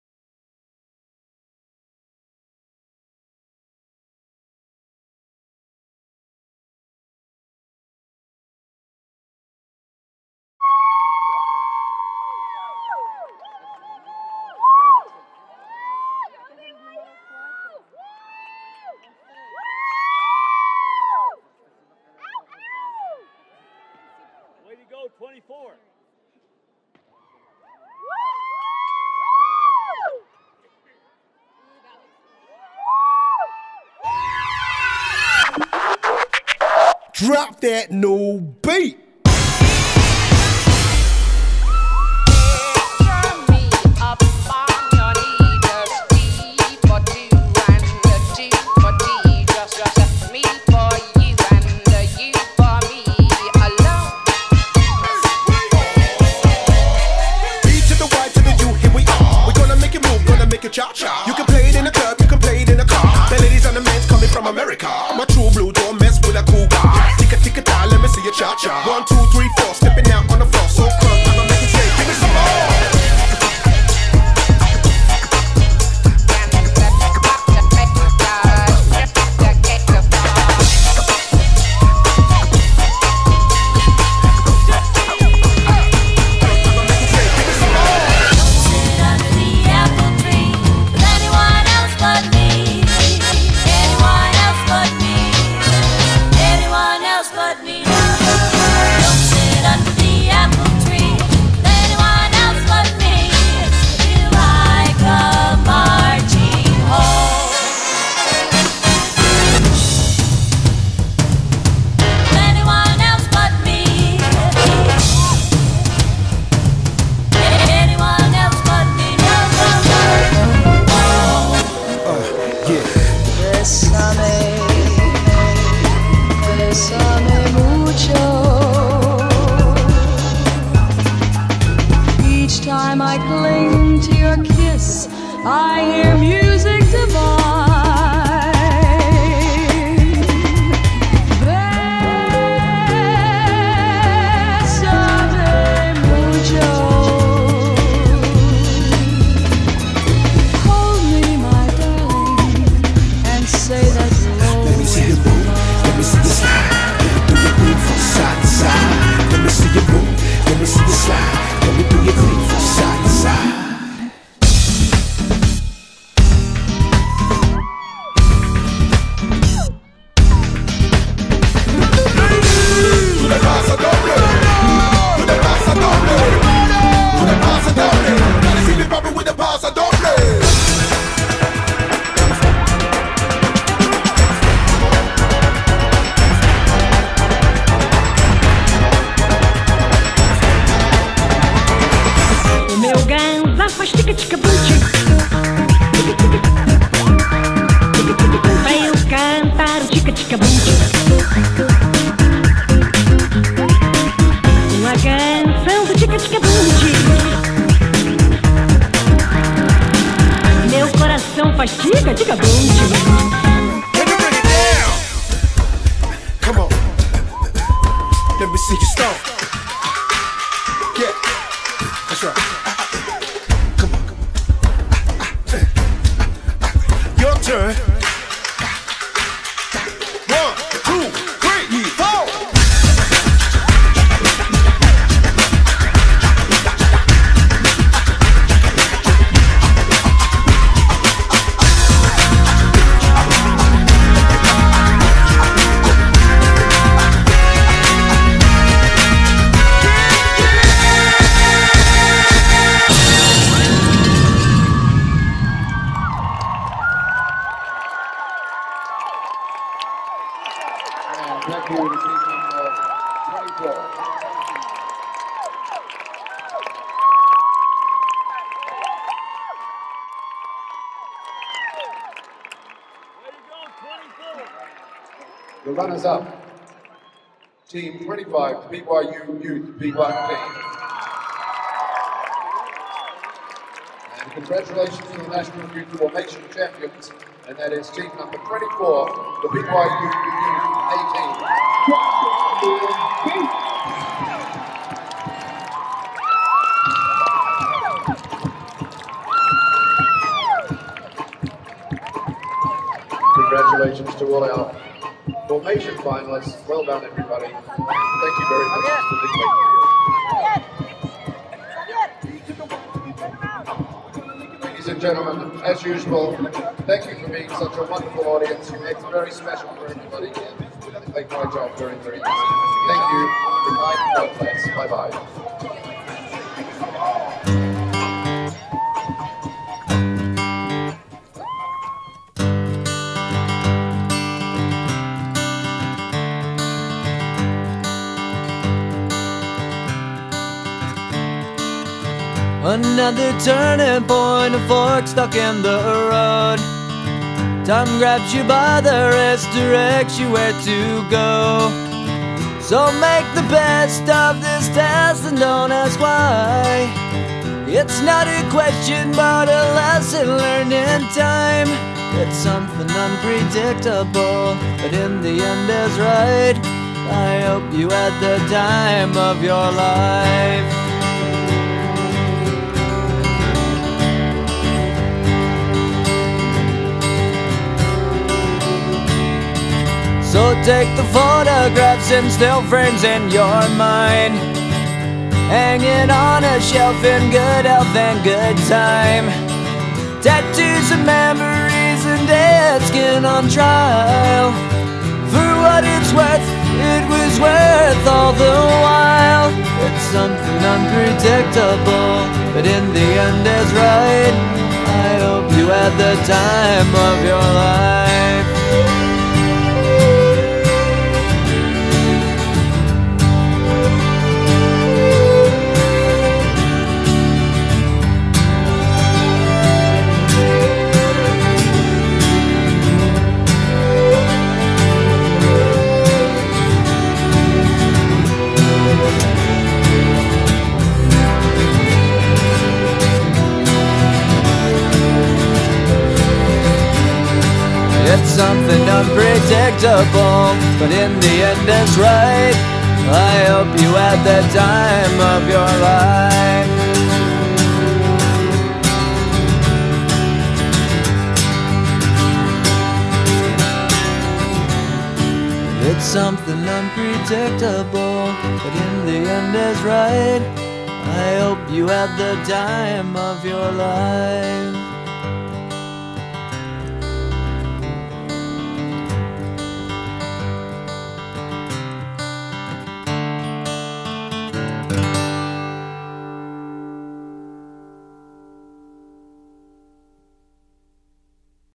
BYU Youth A Team 2015 Latin Medley - Drop That Beat
US national youth latin formation champions, March 14, 2015